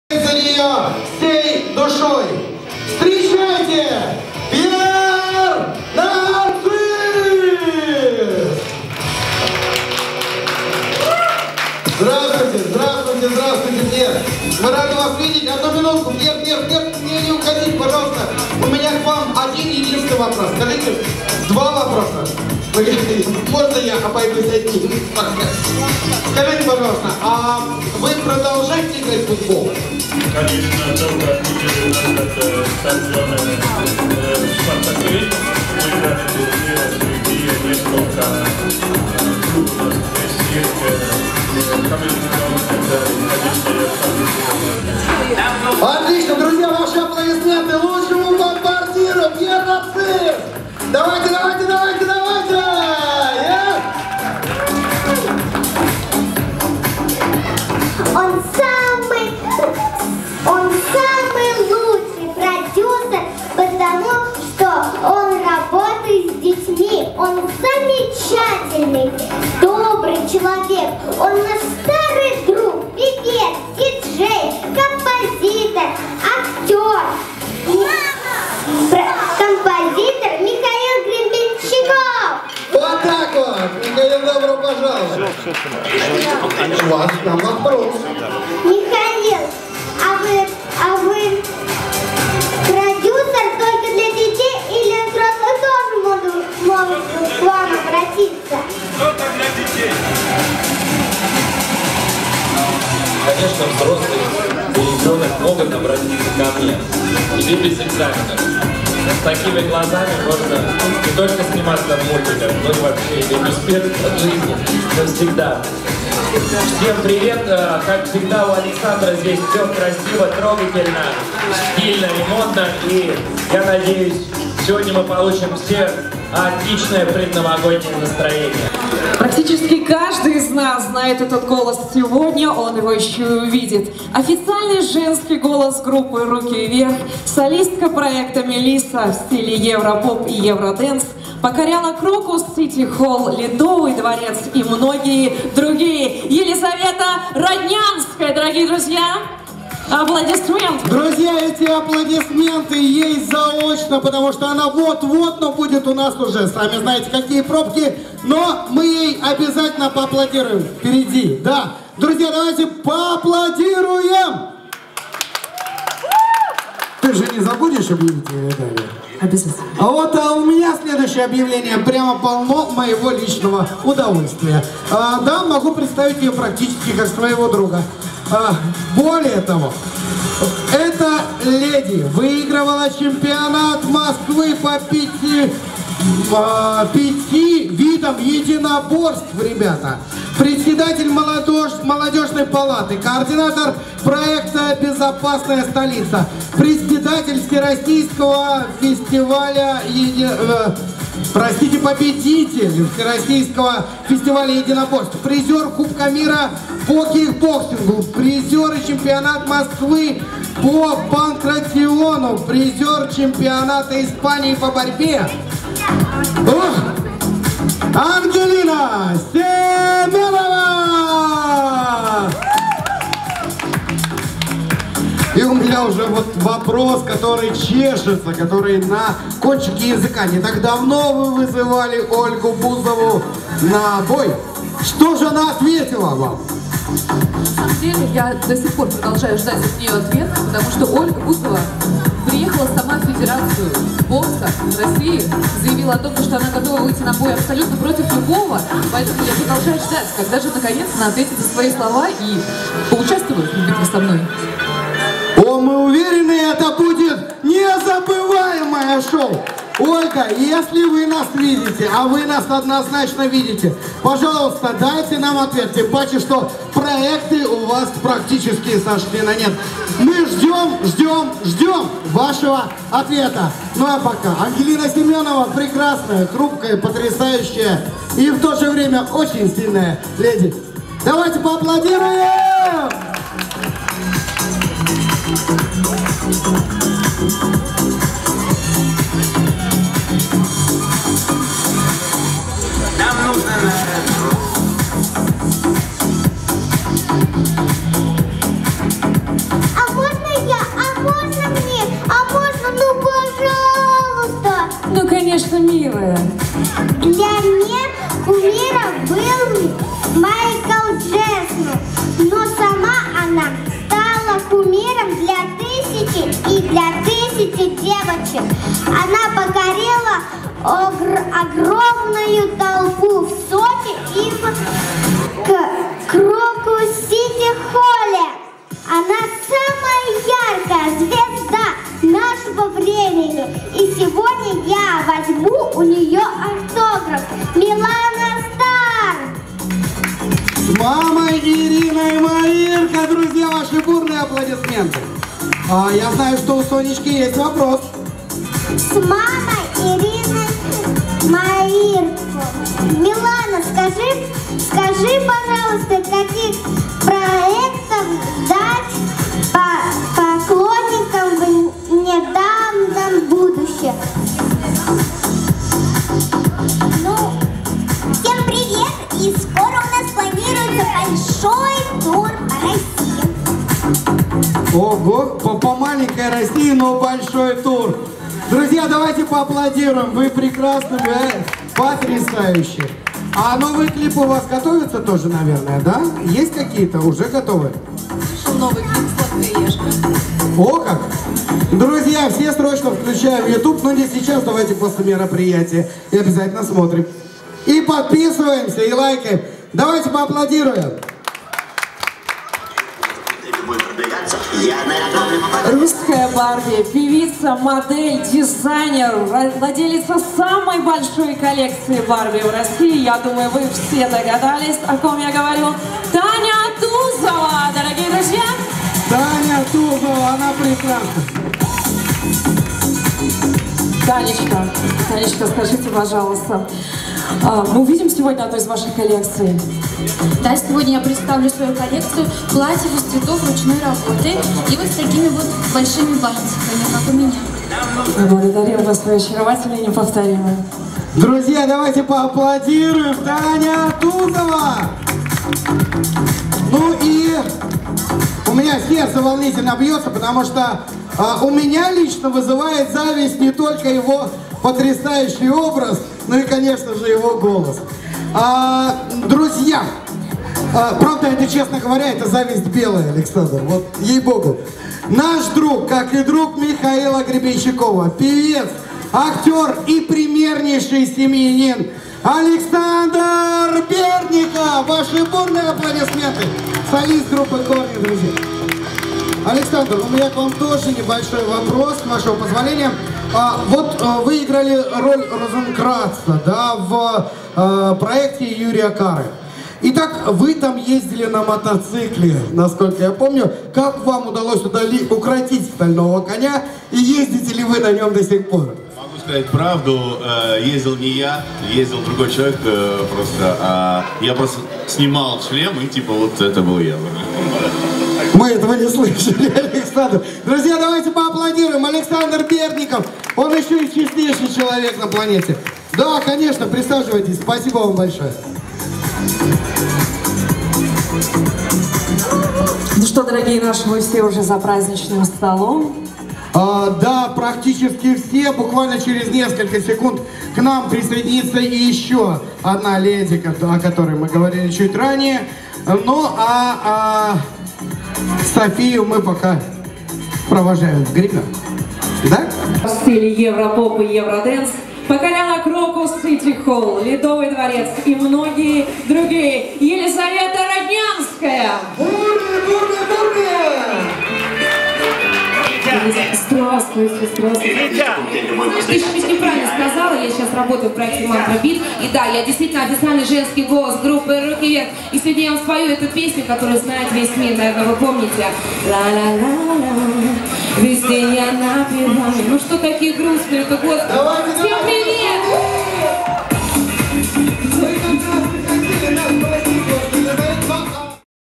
FASHION ЁЛКА 2021 ALEX GROUP - Жюри
6 декабря 2020 в роскошном ресторане «Golden Lotus» @goldenlotus_hall состоялось самое модное событие уходящего года «FASHION ЁЛКА 2020»

Открыл программу фееричный показ дизайнера «ALLADINI» @aladini108 в котором участвовали модели продюсерского центра «ALEX GROUP», финалистки и победительницы всероссийских и международных конкурсов, проводимых «ALEX GROUP». Этот показ окунул всех гостей в настоящую восточную сказку. Роскошь тканей, невероятная фантазия дизайнера и профессиональное воплощение данной коллекции в жизнь подарили всем гостям непередаваемые эмоции и захватили дух.

Организатор мероприятия ПРОДЮСЕРСКИЙ ЦЕНТР «ALEX GROUP»

Была очень насыщенная программа:

🌠Модные показы известных детских дизайнеров

🌠Подготовка образов для моделей профессиональными стилистами и визажистами

🌠Дед Мороз и Снегурочка с подарками для каждого участника

🌠Знакомство и фотосессия с юной звездой MILANA STAR

🌠Выступления известных артистов: Михаила Гребенщикова, Александра Бердникова, Пьера Нарцисса, Елизаветы Роднянской, группы «Куклы Crew» и молодые звездочки Клубкова София, Малахова Лилия, Шапкина Кристина

🌠Знакомство с русской Барби - Татьяной Тузовой и другими звёздами-друзьями нашего продюсерского центра

🌠Профессиональная фотосессия в специальной сказочной фотозоне
🌠Видео и фотосъёмка показов и всего мероприятия

🌠Развлекательная новогодняя шоу программа

🌠Дипломы и подарки от организаторов

Все участники зарядились новогодним настроением вместе с нами и подарили Друг другу настоящий и незабываемый ПРАЗДНИК